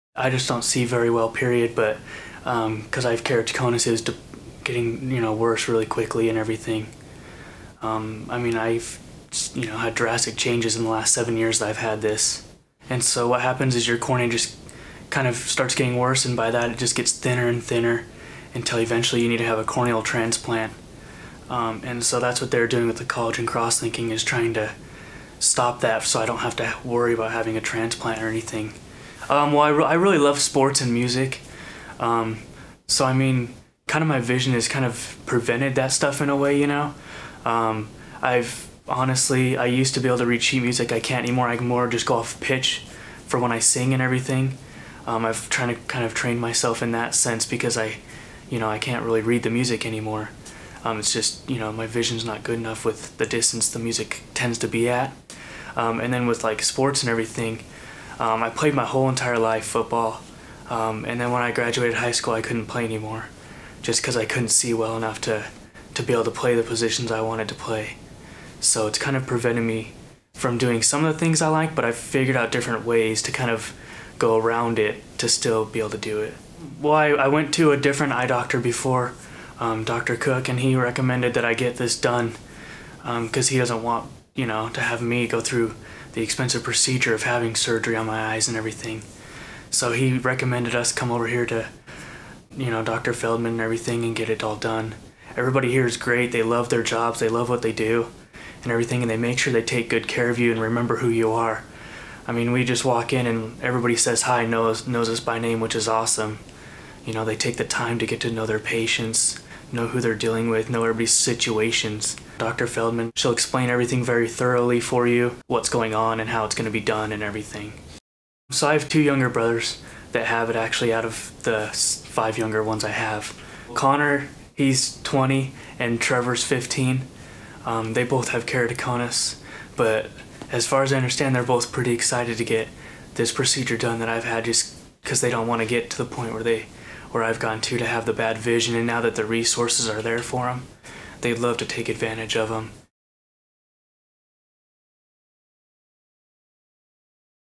New Way To Treat Keratoconus With Corneal Collagen Cross Linking
Keratoconus is a relatively common eye disease, effecting between 1 in 500 and 1 in 2000 Americans. The disease is characterized by a progressive degeneration of the cornea, the front clear window of the eye. The cornea becomes weak, thin, and irregular in shape, resulting in high levels of astigmatism that can interfere with the ability to see clearly. Often, keratoconus patients first require glasses and, as the disease progresses, contact lenses. Often, routine contact lenses do not work but specialized hybrid lenses may give patients good sight. If the condition progresses to a severe level, a corneal transplant may be required.